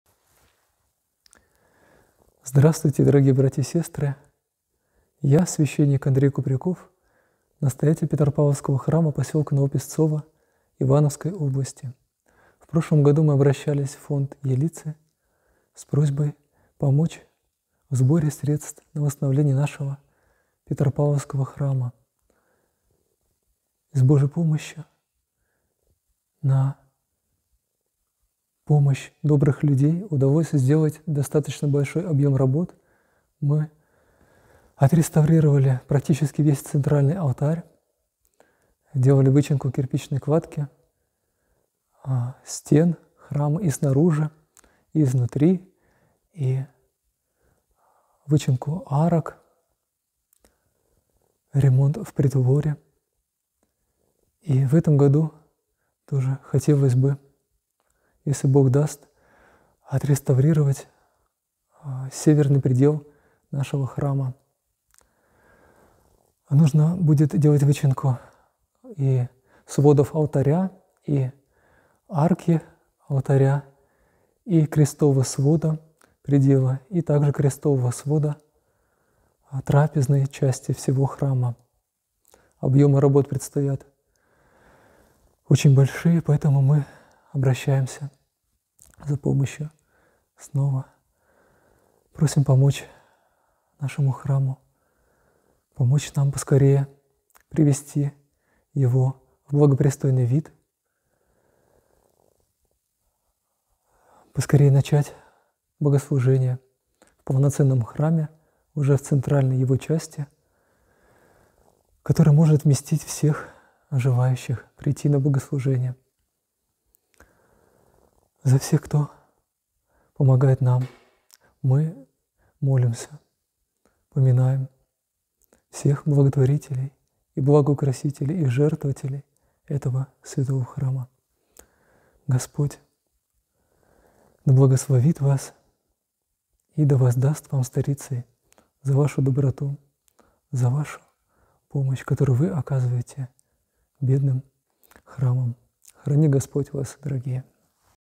Обращение настоятеля Храма свв. Апп. Петра и Павла пос. Новописцово Ивановской области .
За время проведения сбора, собрано и направлено в Храм 170 тыс.рублей. Пожертвовать средства для дальнейшего восстановления можно здесь: